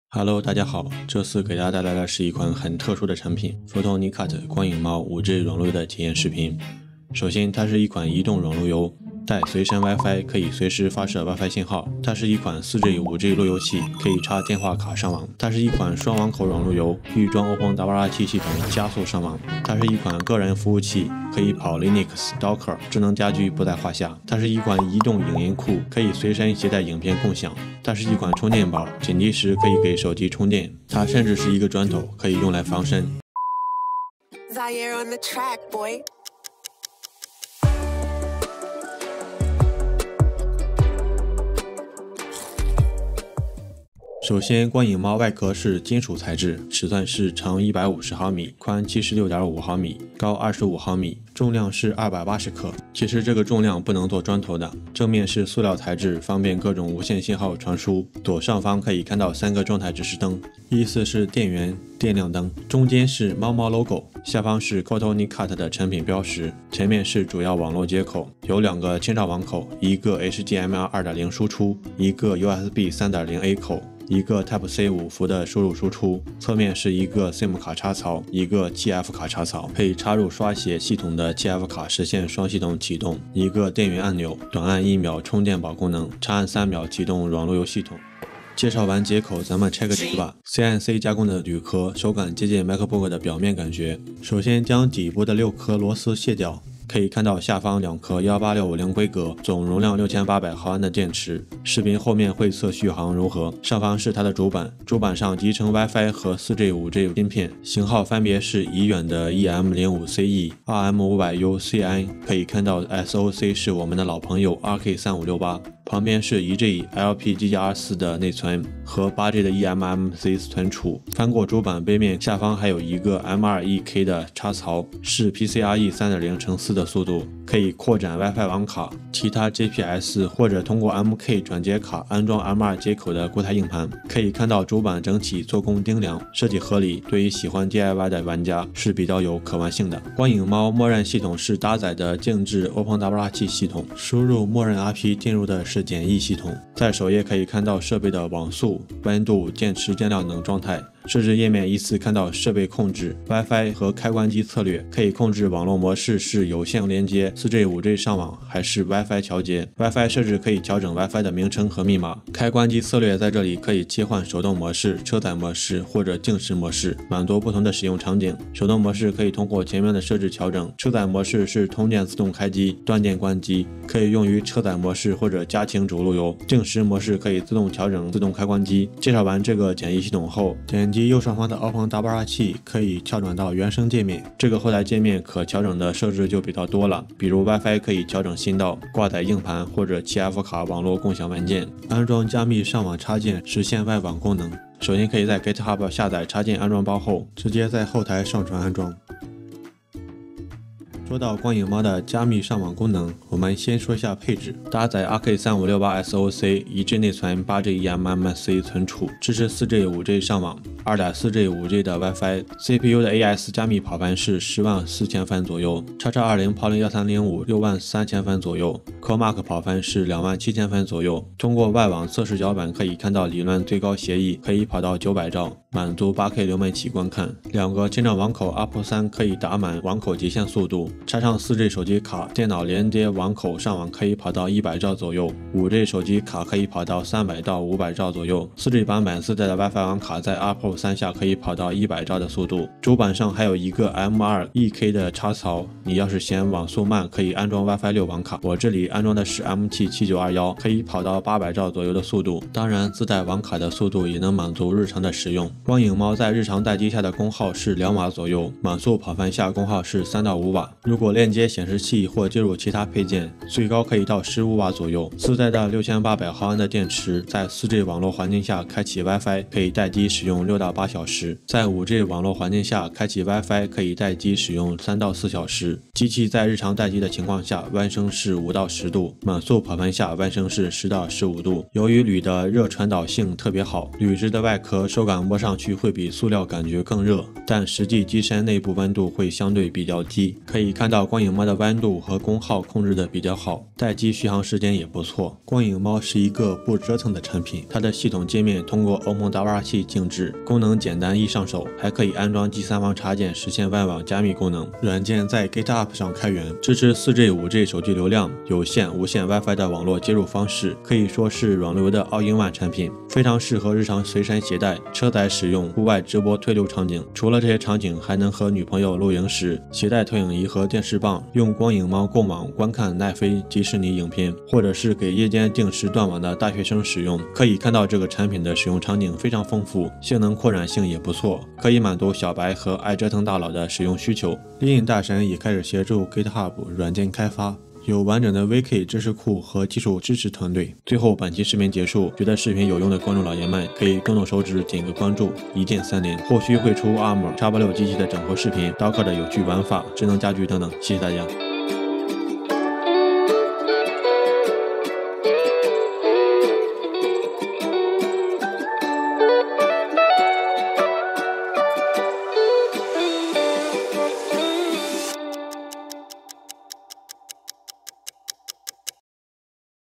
【首发体验】光影猫5G随身OpenWrt无线软路由
这次给大家带来的是一款很特殊产品---photonicat光影猫5G软路由的体验视频。
首先它是一款移动软路由，带随身WIFI，可以随时发射WIFI信号。
它是一款4G/5G路由器，可以插电话卡上网。
它是一款双网口软路由，预装 OpenWrt 加速上网。
它是一款个人服务器，可以跑 linux、docker、智能家居不在话下。
它是一款移动影音库，可以随身携带影片。
它是一款充电宝，紧急时可以给手机充电。
它甚至是一个砖头、可以用来防身 bi